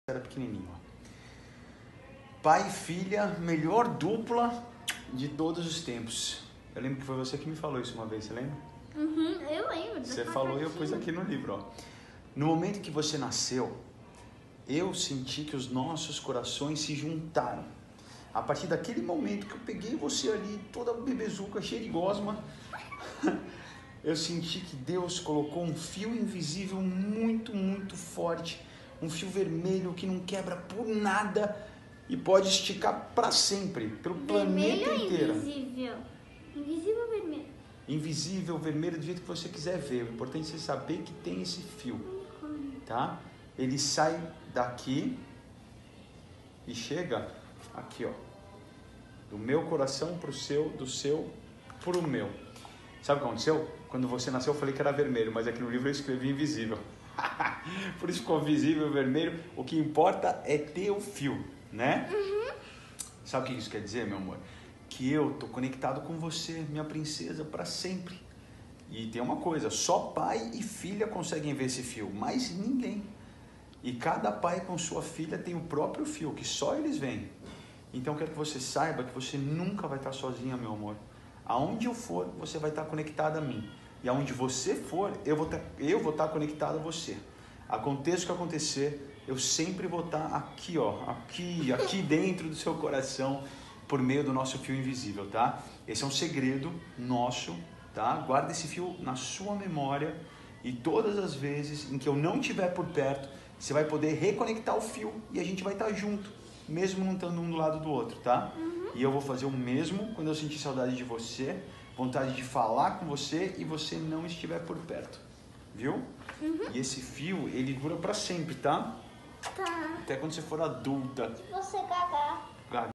Marcos Mion lendo trecho do seu Livro para filha Donatella
Marcos Mion lendo trecho do seu Livro para filha Donatella // Marcos Mion postou um video lendo um dos trechos do seu livro “Pai de Menina”, o apresentador usou a legenda “A história do fio invisível, uma das partes do meu livro que os pais mais usam”. Muito amor envolvido!